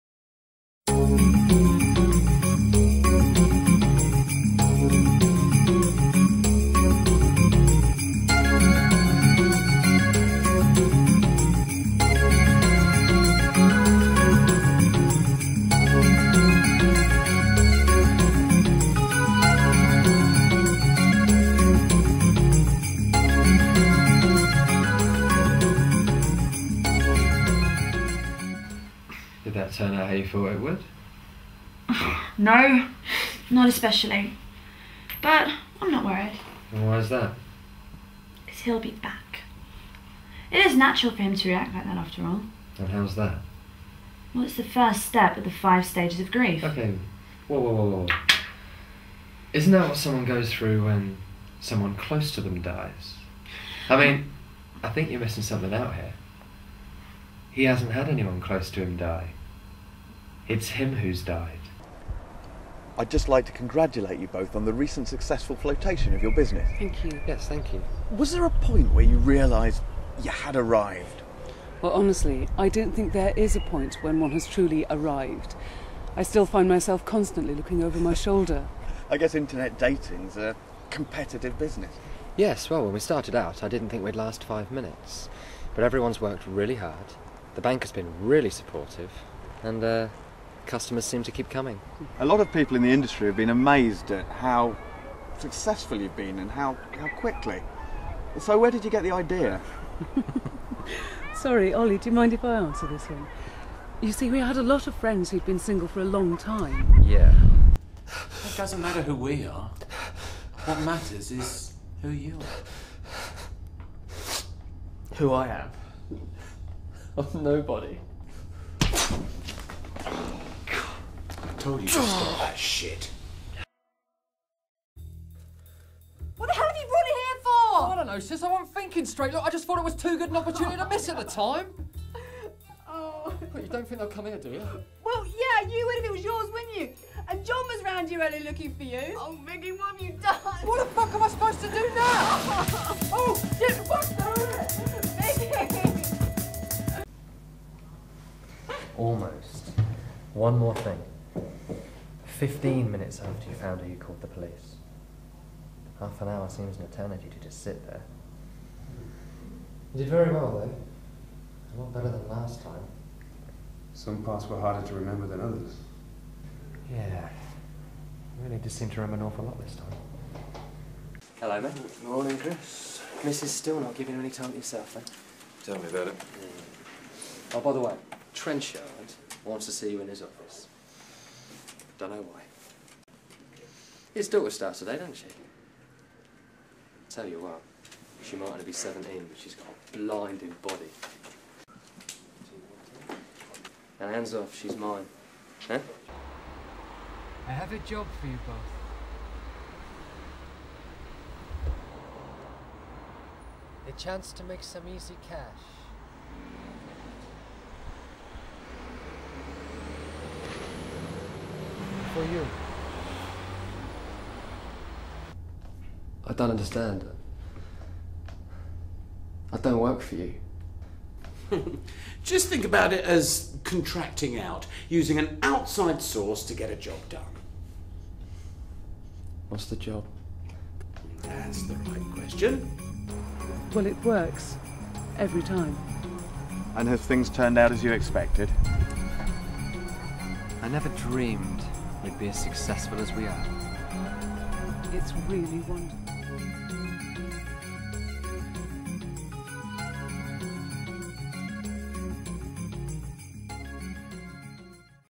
Simon Fox's Showreel
A selection of clips from Simon's recent and past short films; some of which have had success in online competitions and received strong feedback